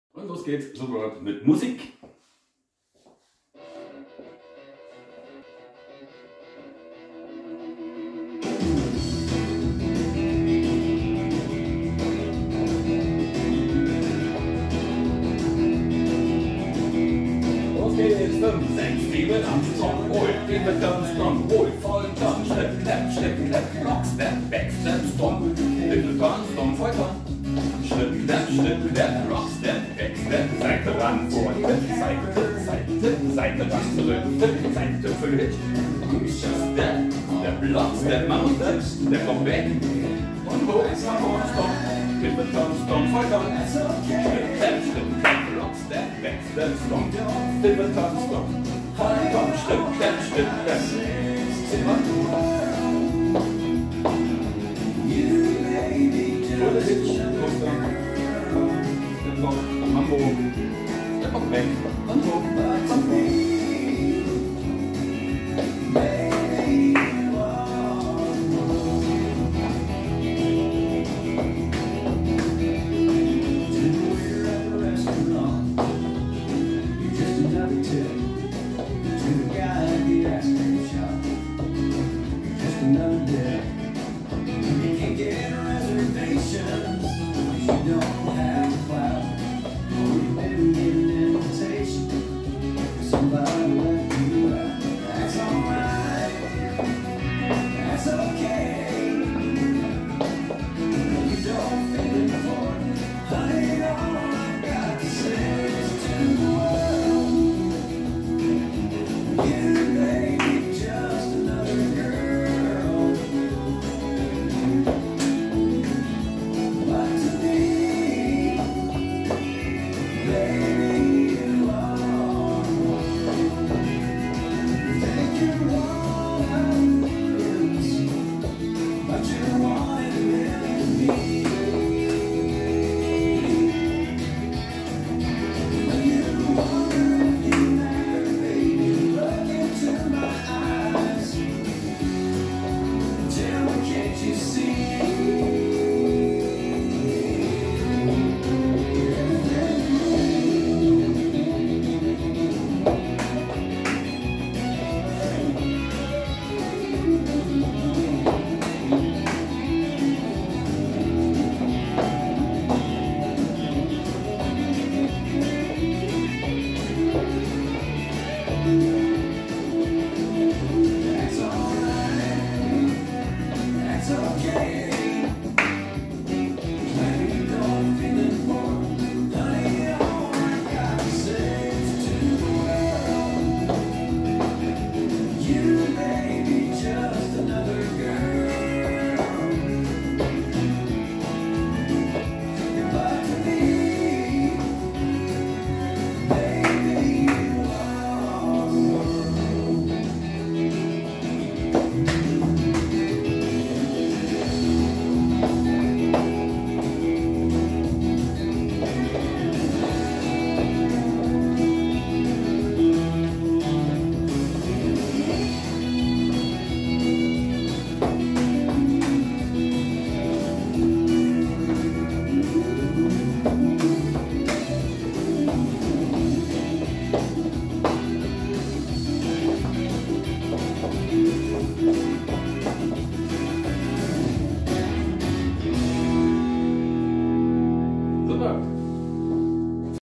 The World DANCE Peer
64 count, 4 wall, improver line dance